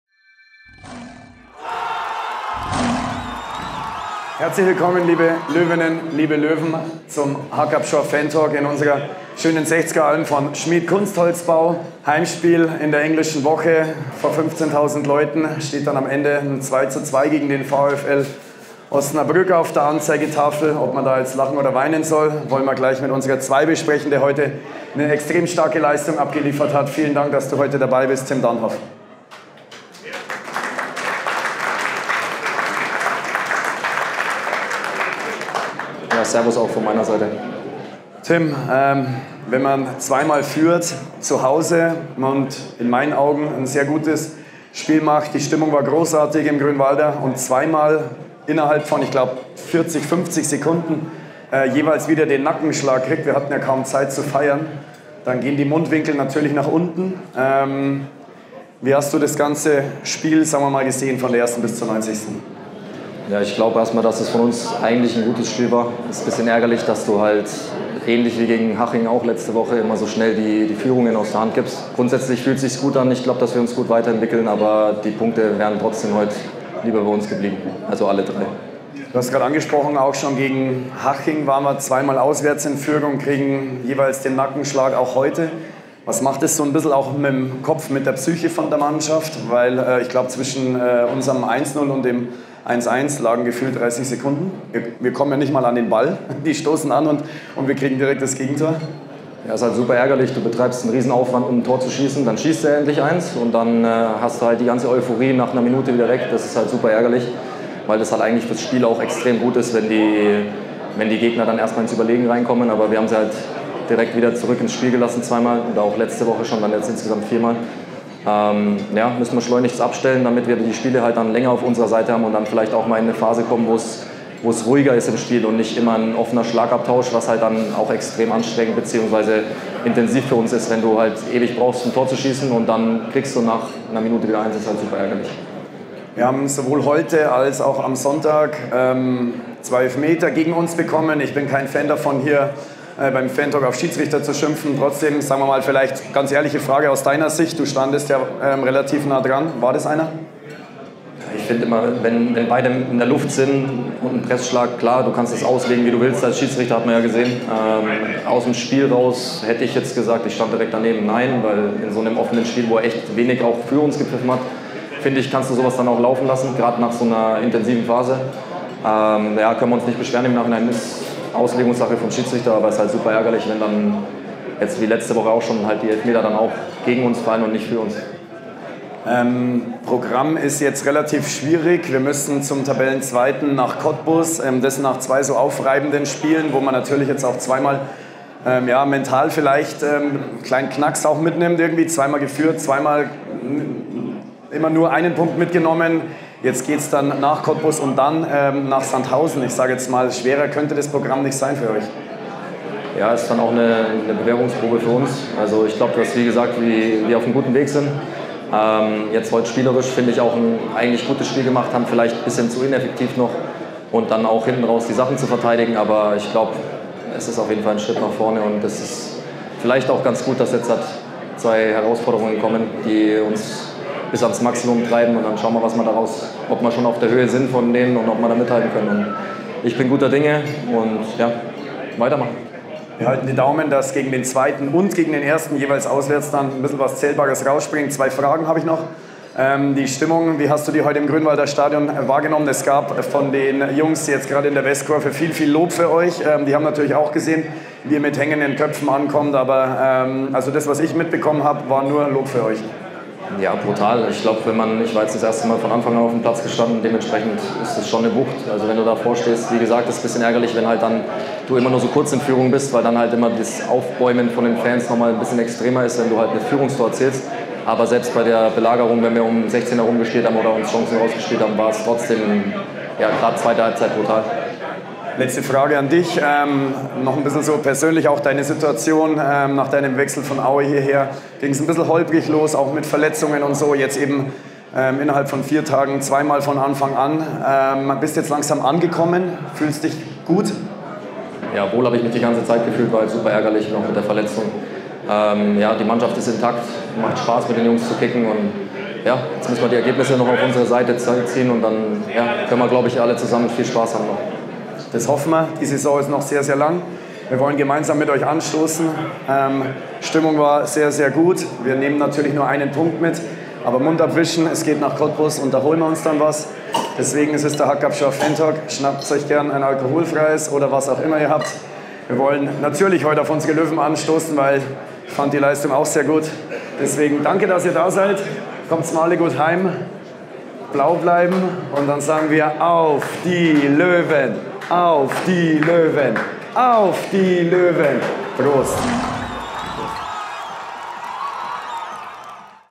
Hacker-Pschorr Fantalk nach M60OSN (23.10.2024).
Sebastian Schäch spricht mit Tim Danhof nach dem 2:2 Unentschieden gegen den VfL Osnabrück über die Partie.